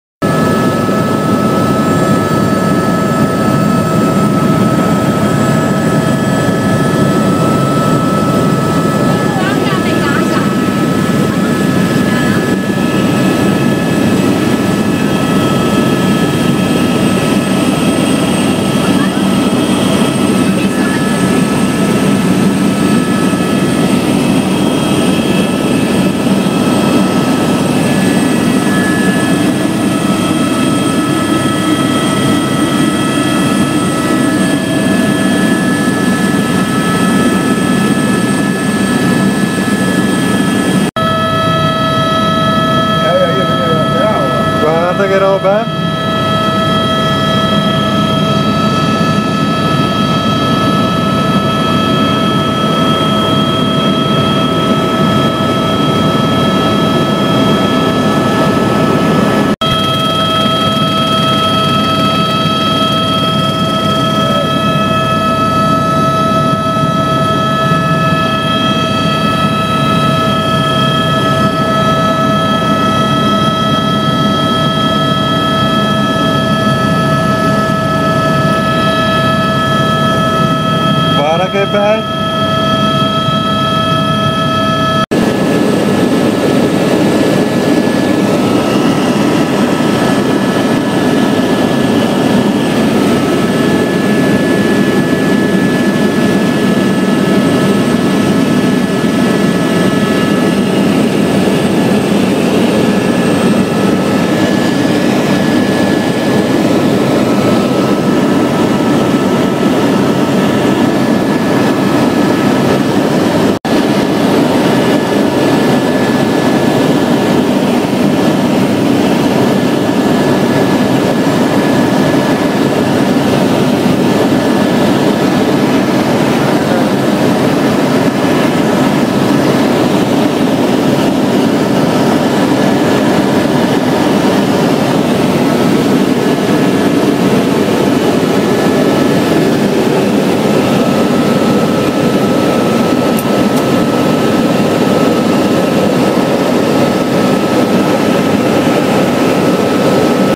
SORVOLO MARMAROLE
Sorvolo delle Marmarole con partenza da Calalzo di Cadore l'8 settembre 2012, video girato con iPHONE da Gianni M.